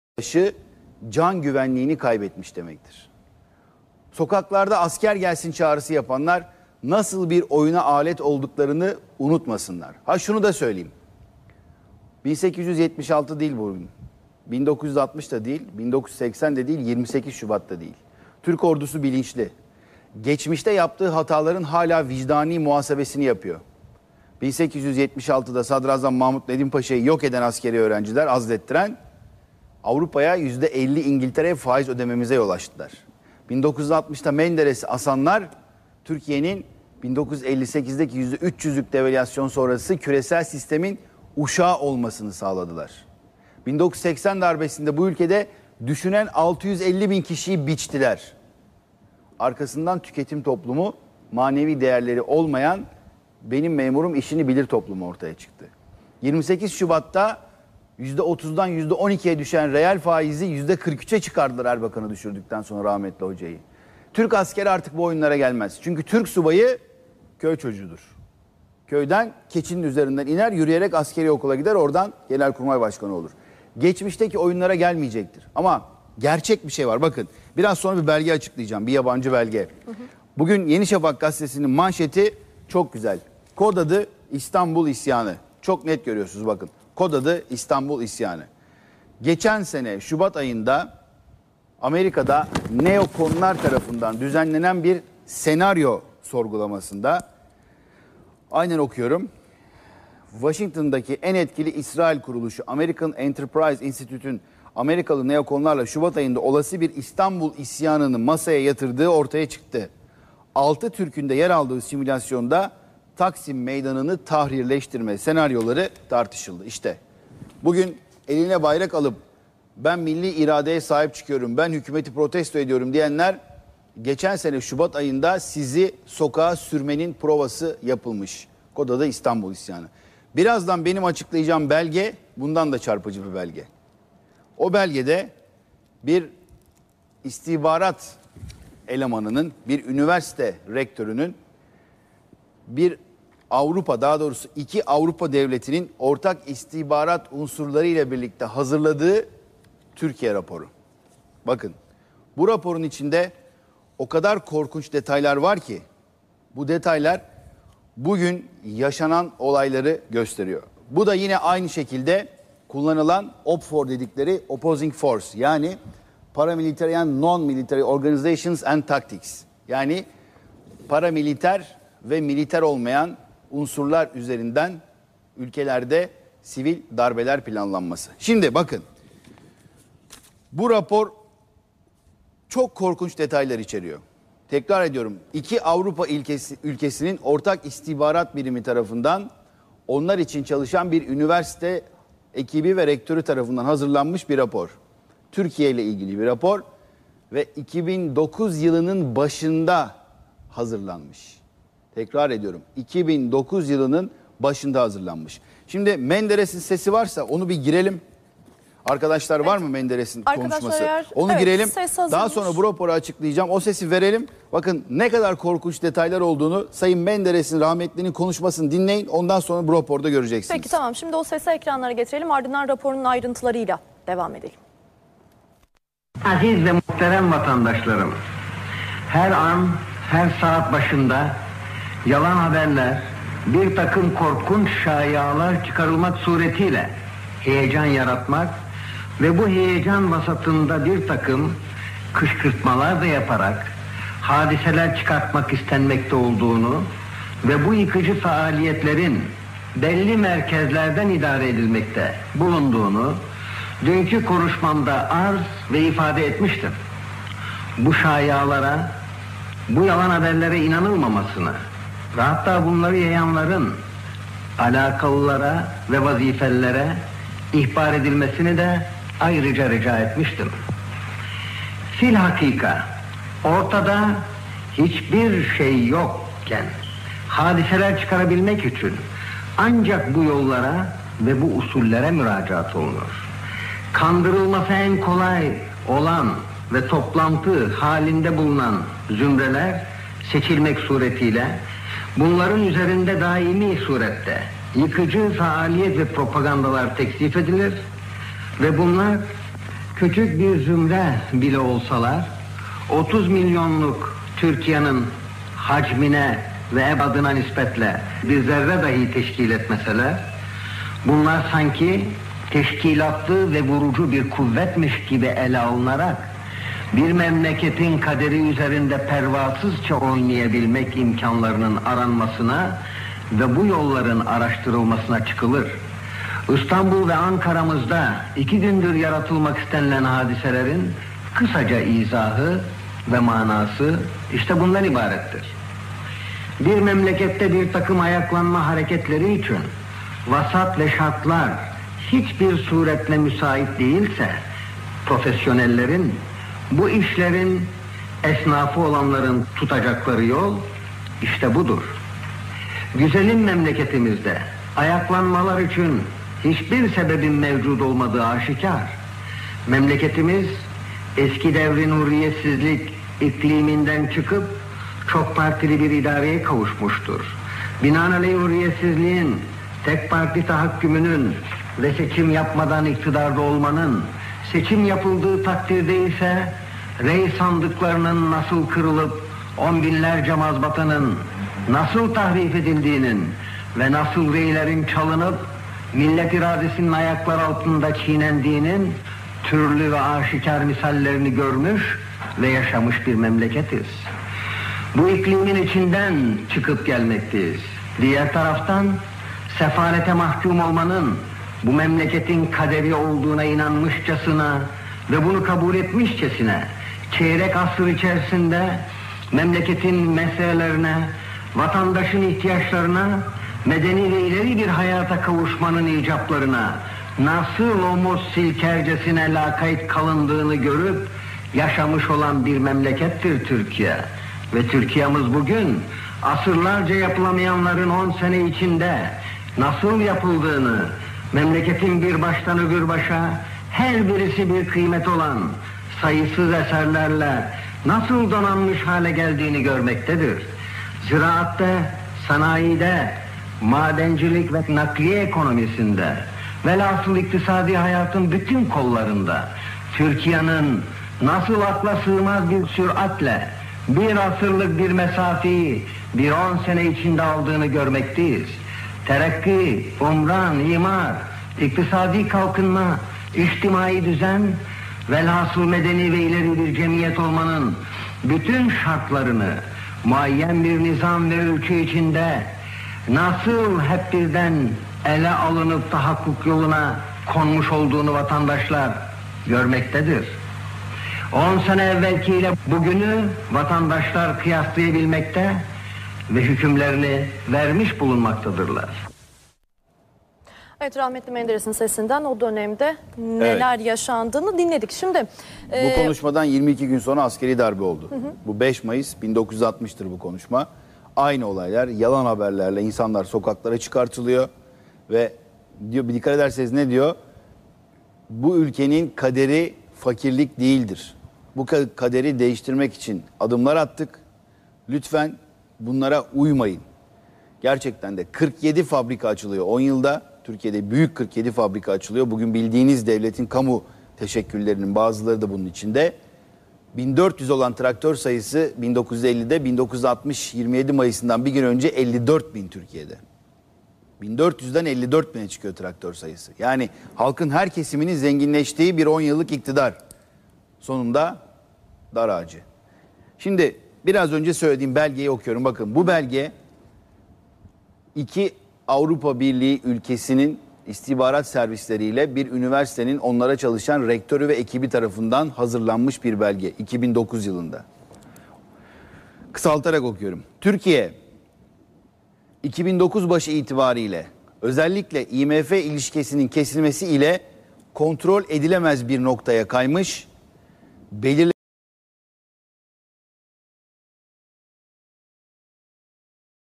İşte o belge 24'te -2-
Yiğit Bulut gündemi değerlendirdi ve iki AB ülkesi ile bir rektör tarafından 2009'da hazırlanan korkunç raporu açıkladı Haber oku Haber oku güncel haberler